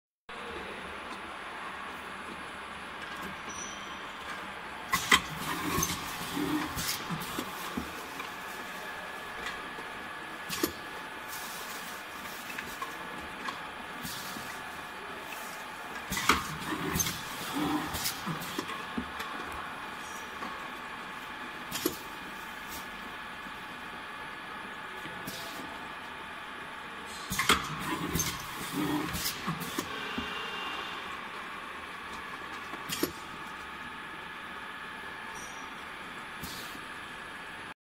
Corrosive filler.anti corrosive filling machine for disinfectant and bleach المضادة للتآكل ملء آلة
CHANGZHOU QINCHENG PACKING MACHINERY CO;LTD

Technical Description

Automatic Anti Corrosive Piston Filling Machine is for viscous corrosive liquid and cream with different bottle or jars. Through PLC, and servo motor, Automatic Anti Corrosive Piston Filling Machine can be changed to fill different volume on touch screen. No bottle no filling. It can be used for acidic toilet bowl cleaner.

Filling range: 50-1000ml
Diameter of bottle: Φ40mm~Φ90mm
Height of bottle: 80mm-230mm
Filling accuracy: +1%(≤200ml)  +0.5%(200-1000ml)
Output: ≤3500BPH
Air resource: 0.6-0.8MPa
Air consumption: 200L/h
Power: 2.0KW/AC220V/380V/50Hz/60Hz Customized
Weight: 600kg
Size(LxWxH): 2000x1050x2300mm